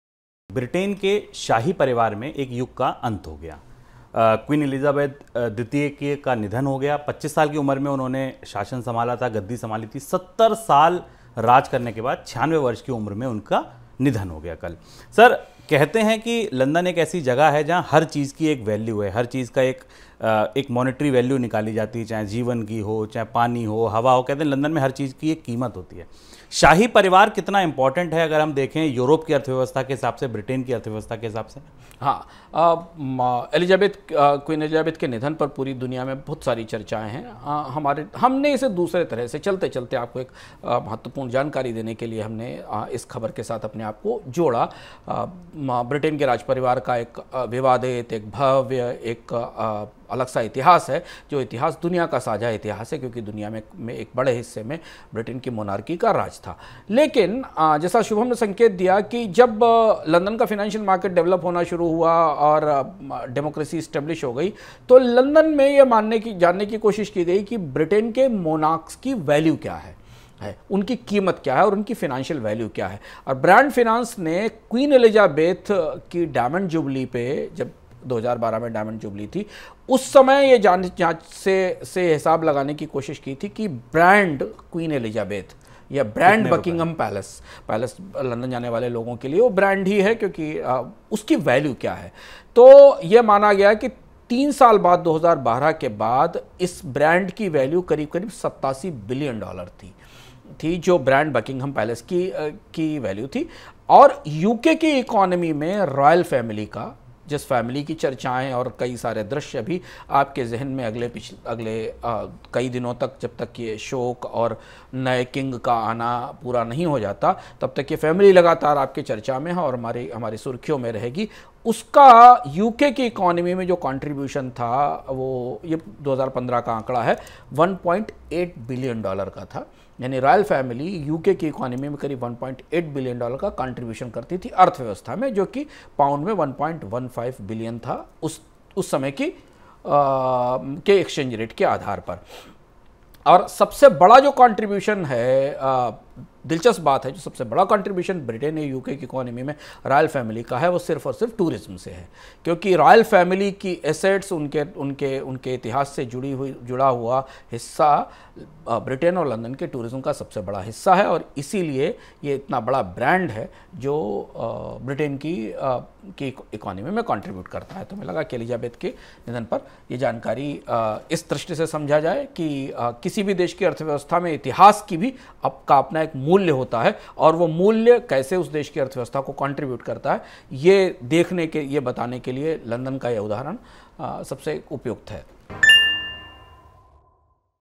Explained: कितना बड़ा ब्रांड है और अर्थव्यवस्था के लिए कितना महत्वपूर्ण है ब्रिटेन का शाही परिवार?
ब्रिटेन की महारानी एलिजाबेथ द्वितीय का गुरुवार को स्कॉटलैंड के बाल्मोरल कैसल में निधन हो गया. एक युग का अंत हो गया. लंदन में हर चीज की एक कीमत होती है. सवाल ये कि कितना बड़ा ब्रांड है और अर्थव्यवस्था के लिए कितना महत्वपूर्ण है ब्रिटेन का शाही परिवार?